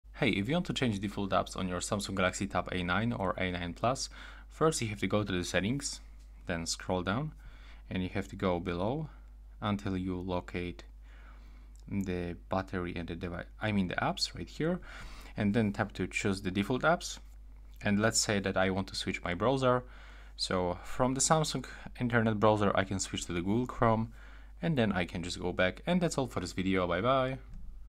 Samsung Galaxy Tab A9/A9+ Change Default Apps
Customize your Samsung Galaxy Tab A9 or A9+ experience by learning how to change default apps with this helpful video guide. Join me as I walk you through the steps to set your preferred apps as defaults, ensuring a personalized and seamless tablet usage.